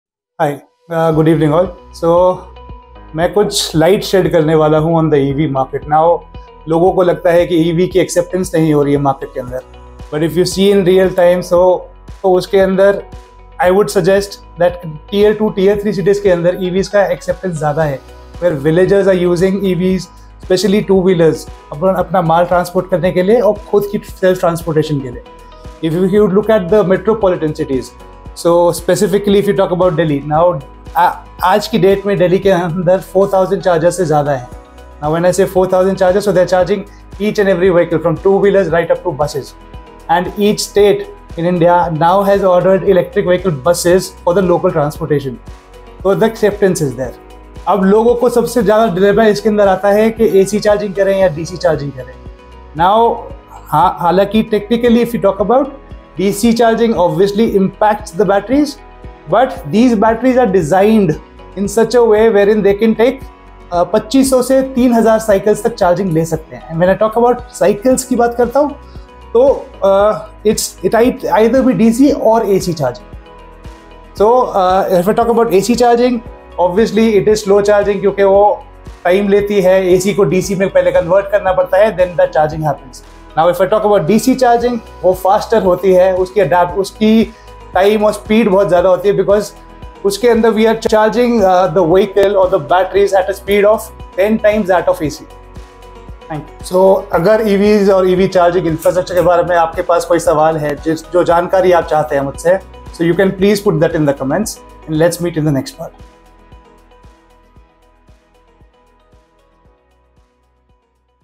Exploring the Electric Vehicle Revolution: Insights from Shashank, Our EV Expert
Join us as our EV expert delves into the dynamic world of electric vehicles (EVs). In this video, we shine a light on the booming EV market, discussing the latest trends, technological advancements, and the future of sustainable transportation. From EV adoption rates to the environmental impact, we cover it all. Don't miss out on this electrifying conversation!